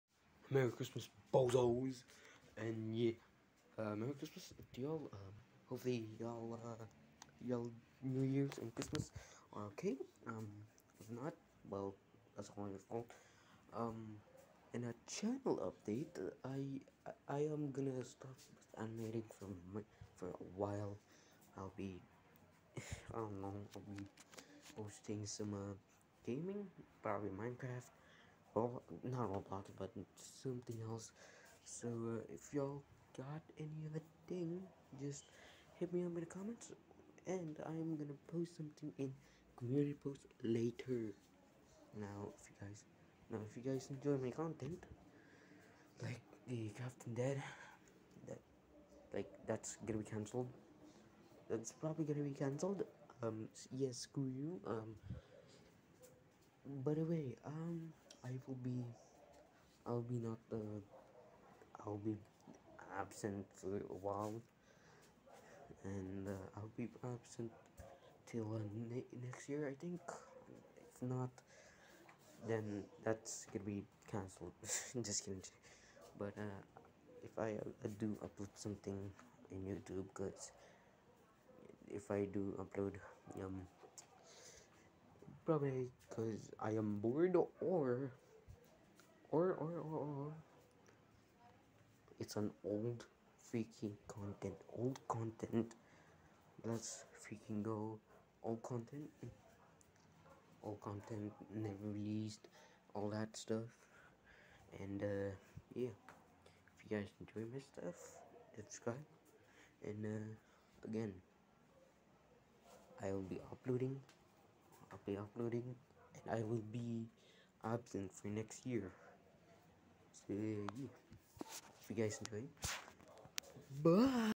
channel updates and MERRY CHRISTMAS and join the discord
yeah but I'll be absent for next year i think but I'll be uploading old/un release content maybe I'll edit them to creeper but yeah if im bored I'll upload a vid cuz yes and i promise i actually promised I'll be live

More people/friends are in the bottom in the description